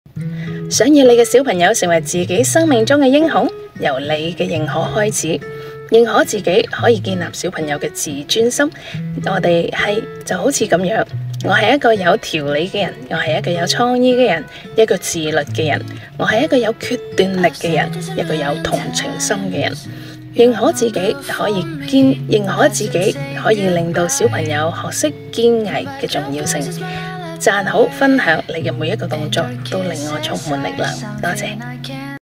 感恩今天：與孩子共享認可自己的內在力量 - 100日挑戰十四天
讓你的孩子成為自己生命的英雄，從你的認可開始。
你有冇覺得自己嘅孩子對自己嘅形象感到不滿或不自信？
認可自己能建立孩子的自尊心。